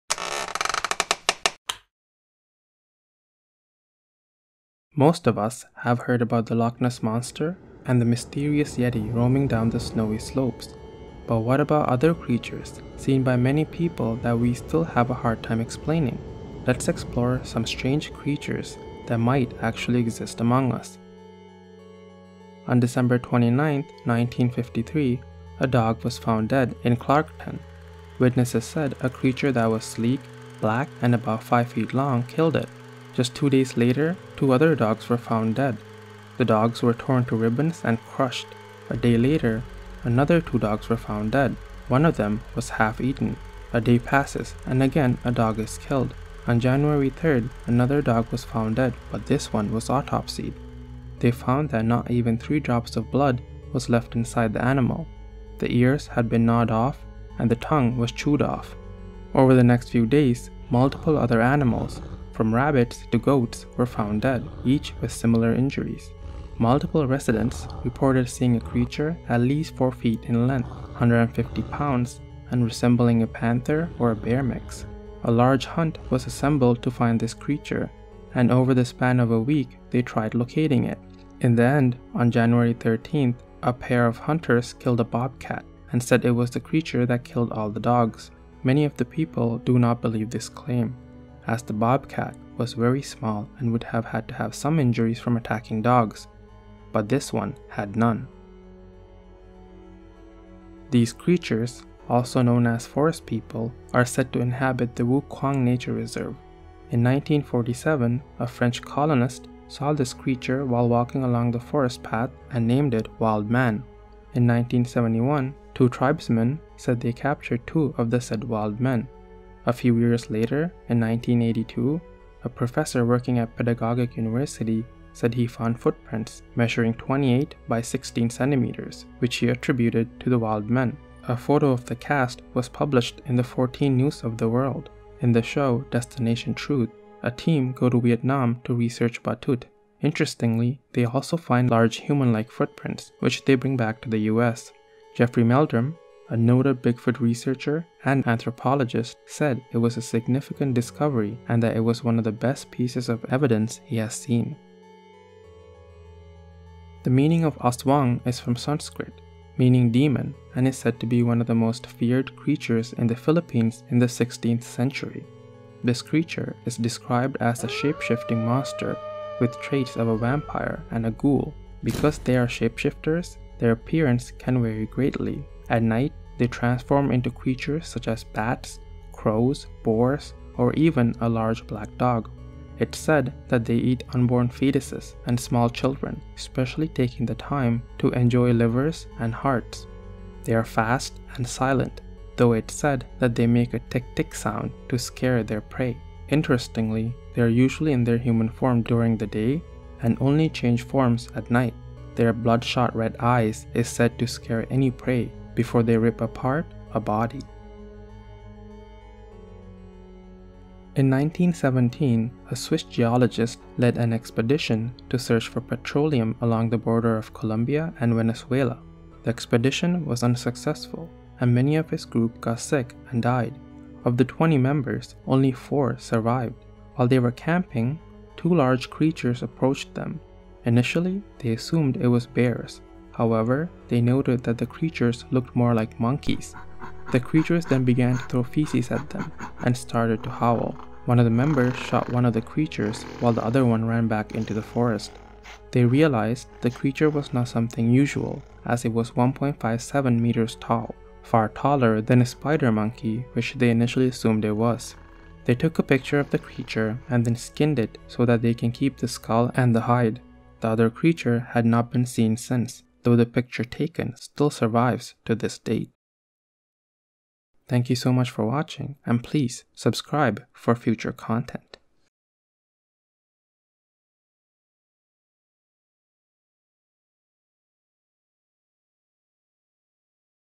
4 Mysterious Creatures That Might Actually Exist (Cryptids)
Let’s explore some strange creatures that might actually exist among us.

Beast of Bladenboro
On December 29th, 1953, a dog was found dead in Clarkton. Witnesses said a creature that was, “sleek, black, about 5 feet long. ” killed it.

Batutut
These creatures, also known as forest people are said to inhabit the Vu Quang nature reserve. In 1947, a French colonist saw this creature while walking along the forest path and named it wild man.

Aswang
The meaning of Aswang is from Sanskrit, meaning demon and is said to one of the most feared creatures in the Philippines in the 16th century.

De Loys’ Ape

In 1917, a Swiss geologist faced a large monkey like creature and took a picture.